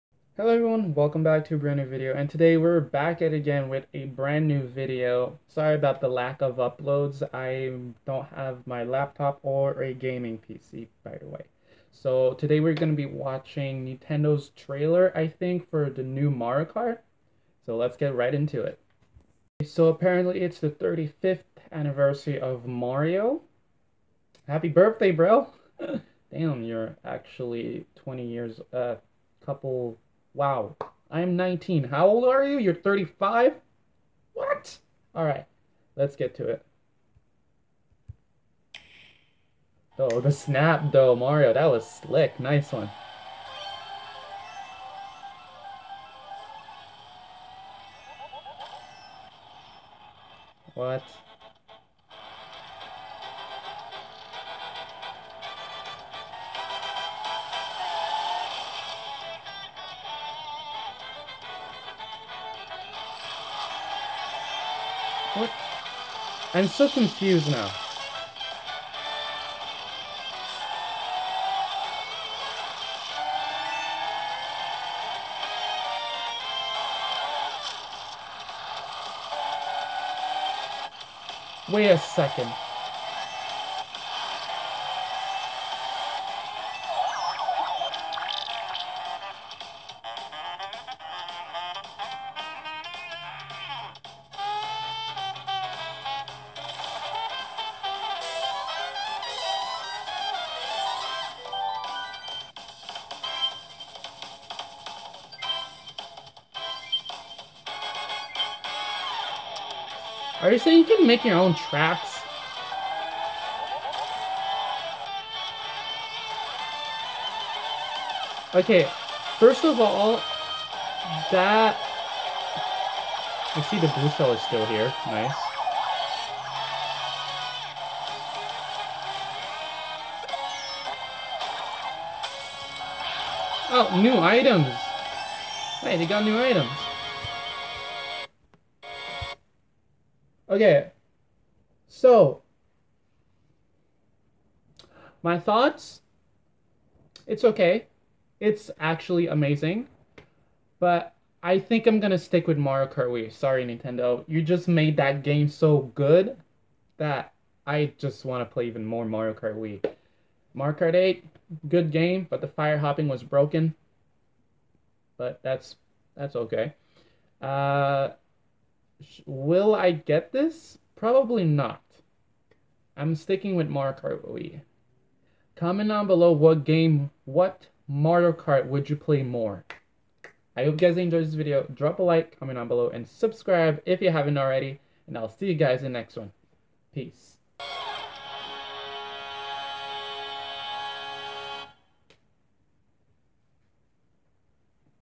A New Mario Kart Game?
Hi Everyone Today We Take A Look At The New Mario Kart Game Comming Soon!?

Watch Intill The End These Are My Thoughts On Mario Kart Live Home